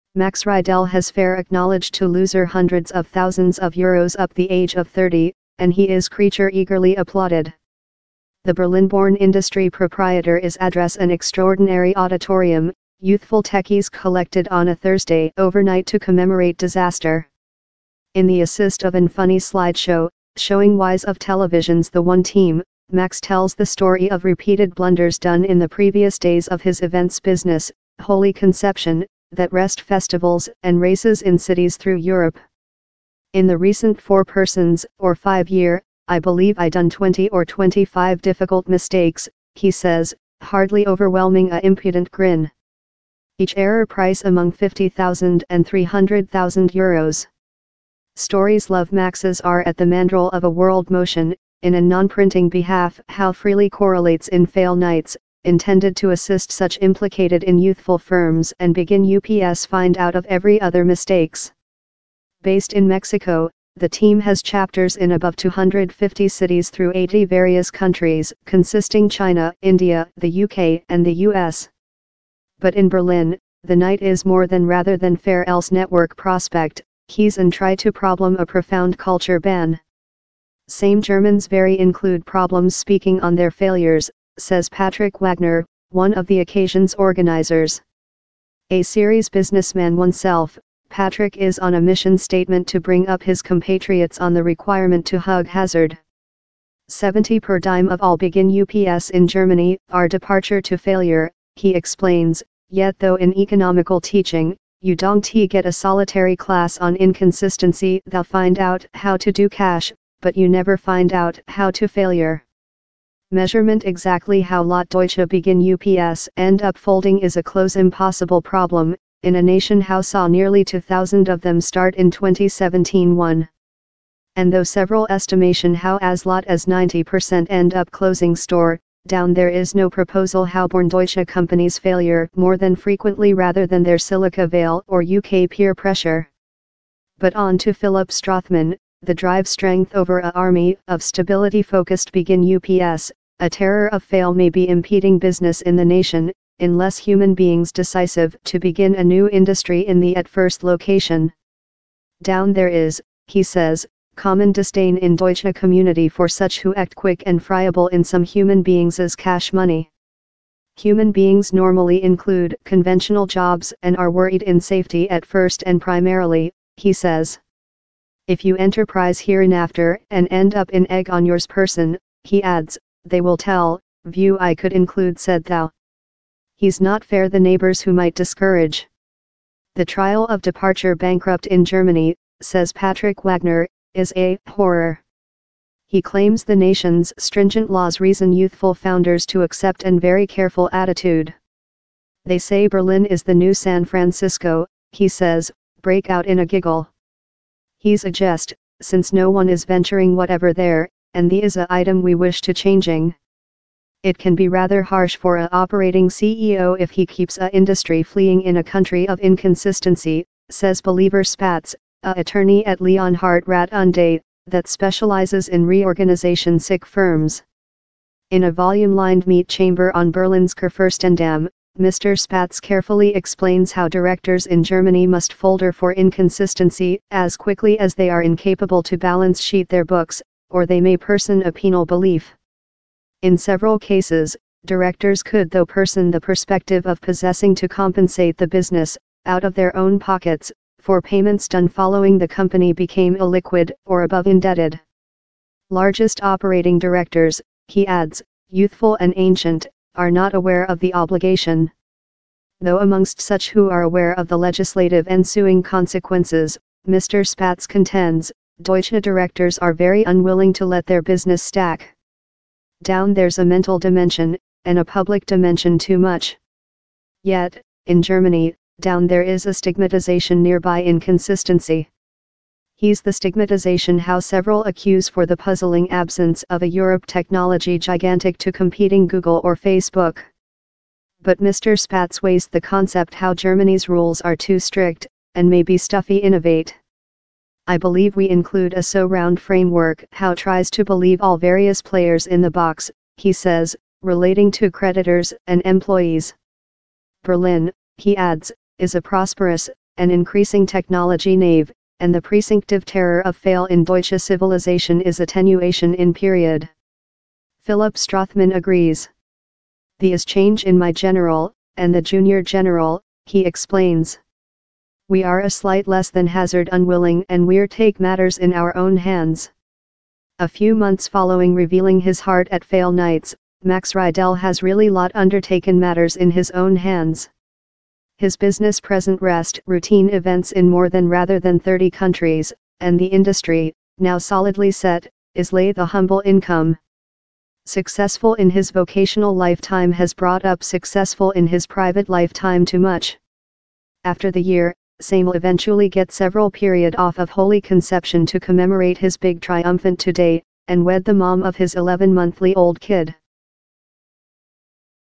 The Deutsche Entrepreneurs Celebration Their Mistakes
Talking about failure is taboo in Germany, and it could be holding business back.